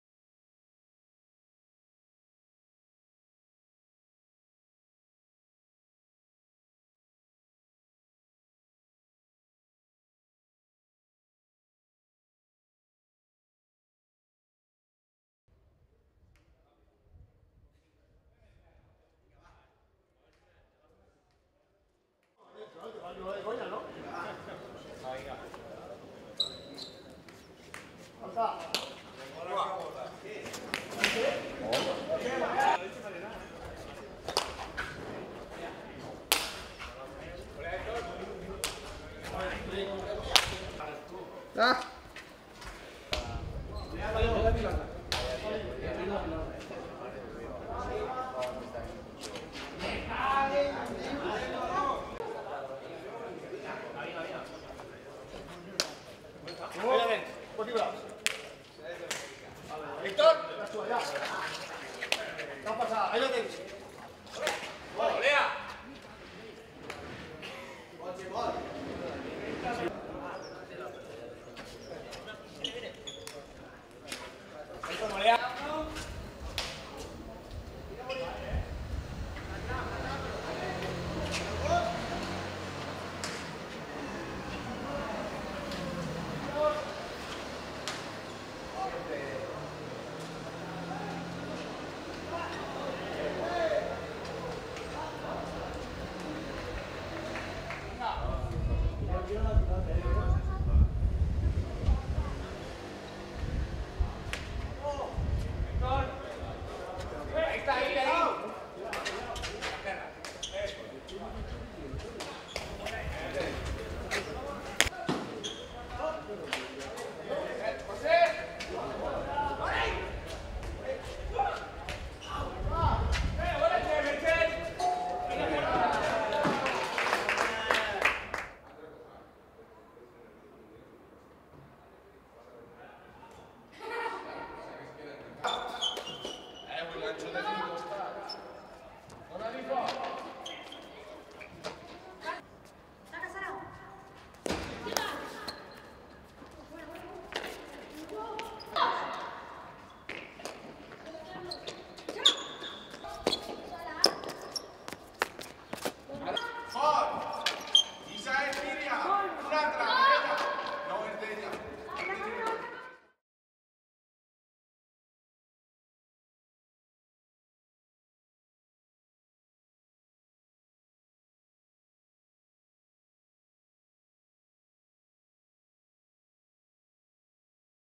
Juego de la Galotxa
• Formas de socialización colectiva y organizaciones

La 'Galotxa' es una modalidad del juego de la conocida 'pilota valenciana'. Desde antiguo se ha jugado en la calle y normalmente se juega entre equipos de diferentes pueblos. En este juego siempre han participado personas de diferentes estamentos sociales, desde alcaldes o sacerdotes, pasando por agricultores u obreros. En la localidad valenciana de Foios se trabaja con los jóvenes para que este juego no se pierda y se siga practicando en el futuro.